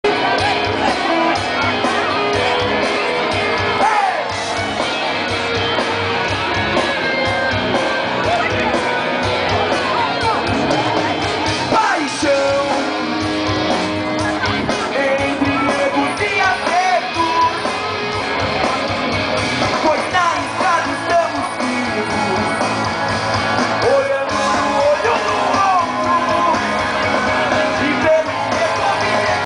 Paraquedas voando - Promoção Nescau na MegaRampa no Anhembi
A organização estava distribuindo camisas do evento através de pequenos paraquedas para os espectadores da arquibancada. Tudo isso ao som de Rancore